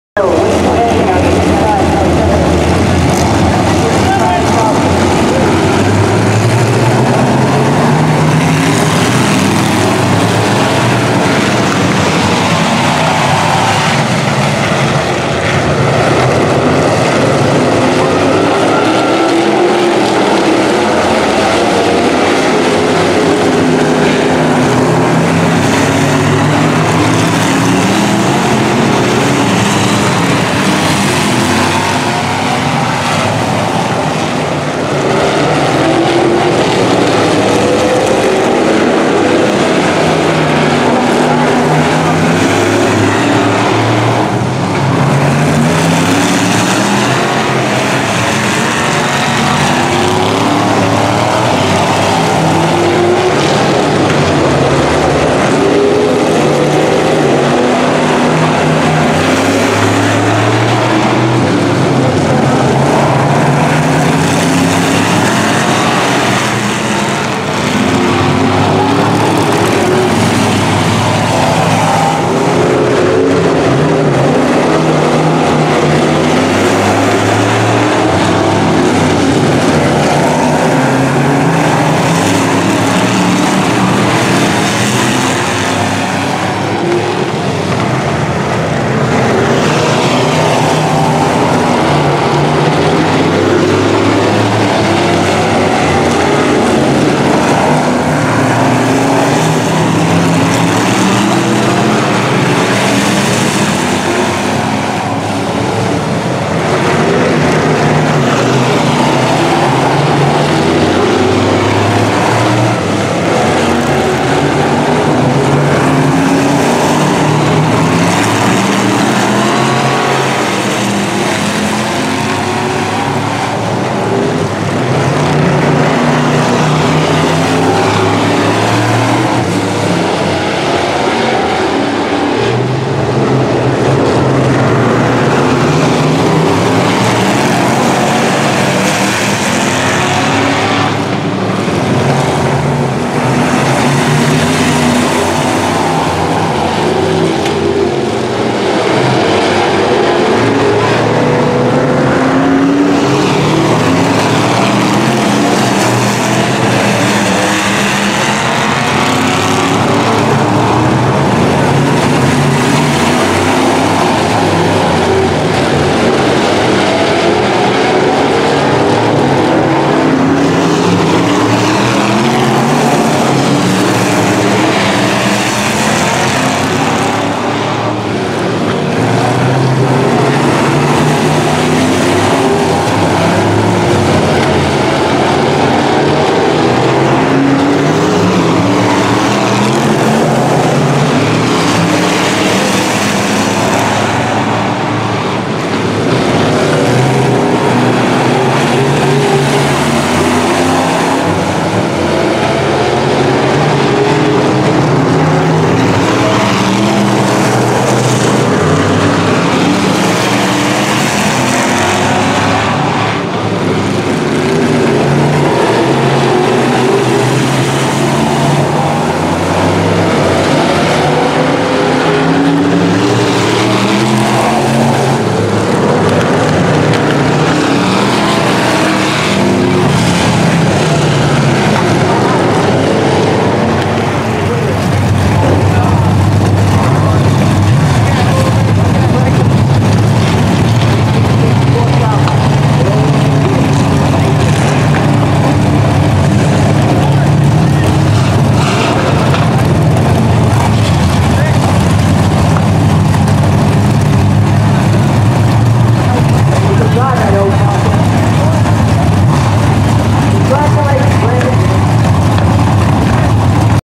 Winder Barrow Speedway NVRA Race One 8/19/17
Great seeing Zack & Judy Minielly and all the NVRA Race Cars at the Baja!